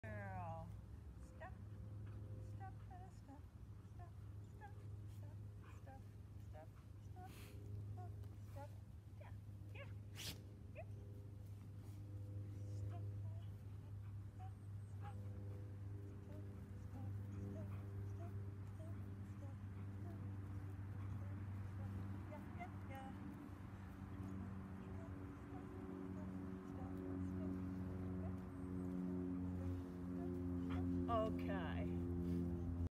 Fun Dog Tricks: High Steppin’ Side Steppin’ Lark
Welsh Springer spaniel Lark adds the Side Pass to her dancing steps.  Watch her step high, step sideways right in tune with me.